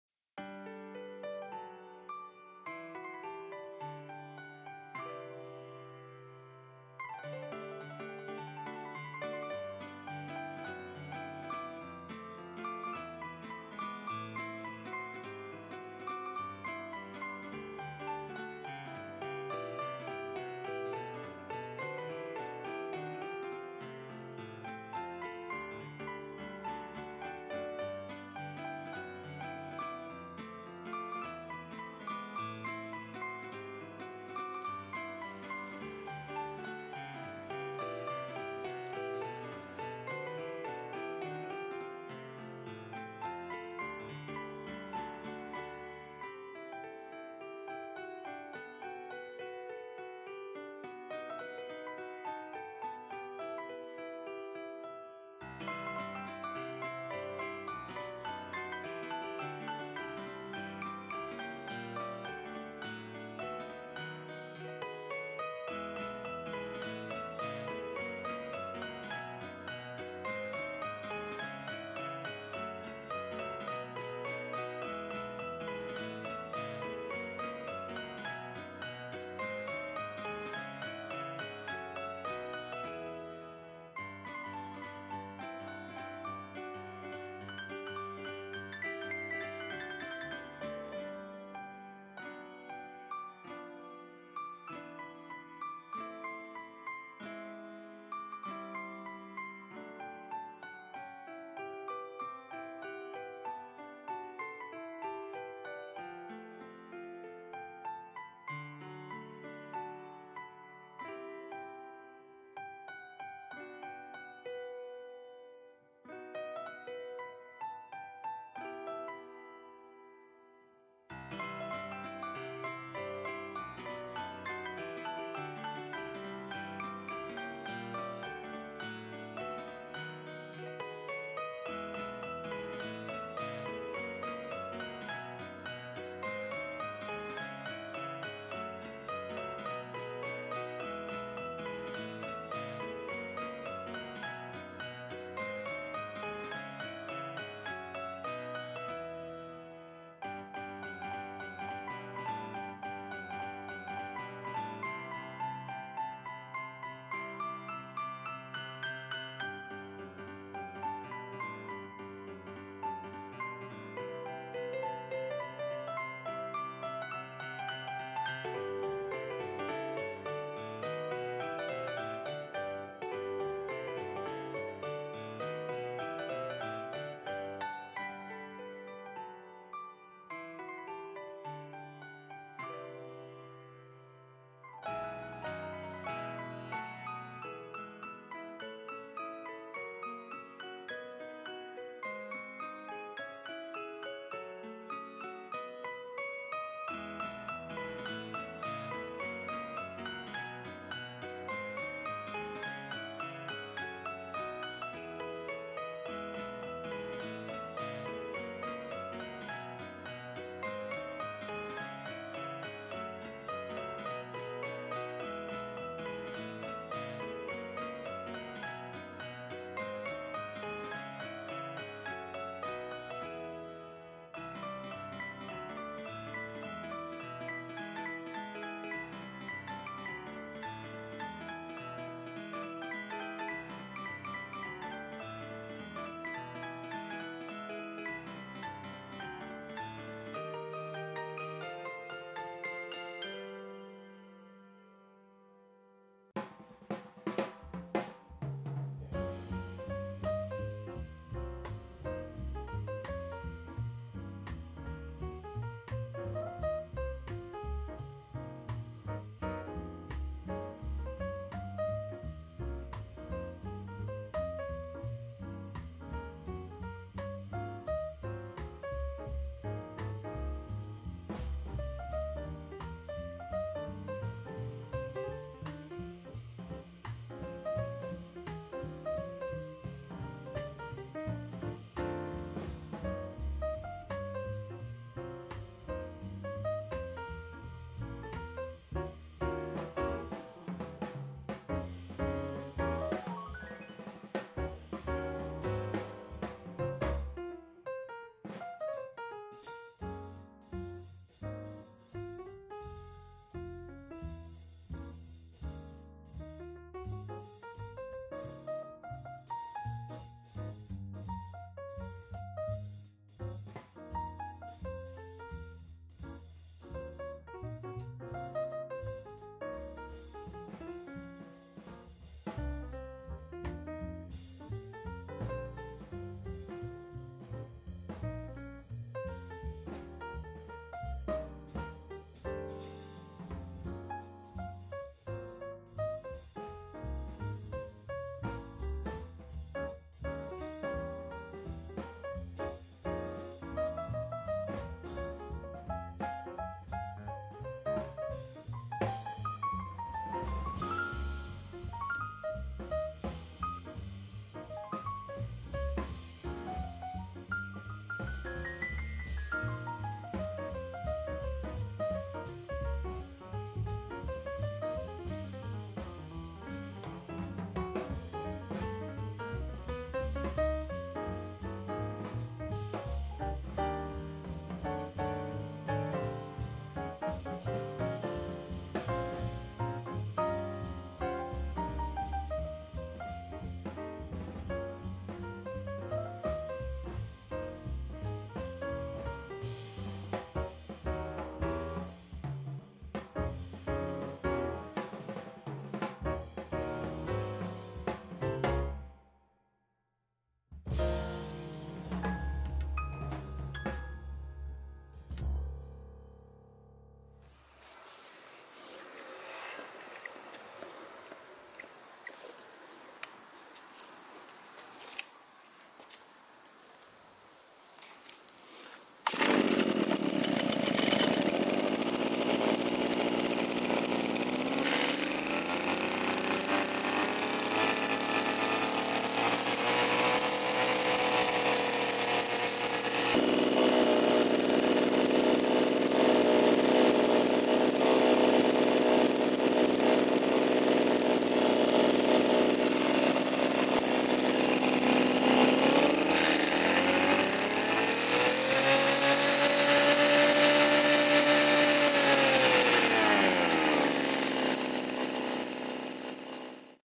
草刈り機キャブレター(ダイヤフラム式)分解洗浄 三菱ＴＬ261
冬の間使っていなかった草刈り機のキャブレターメンテナンスを行いました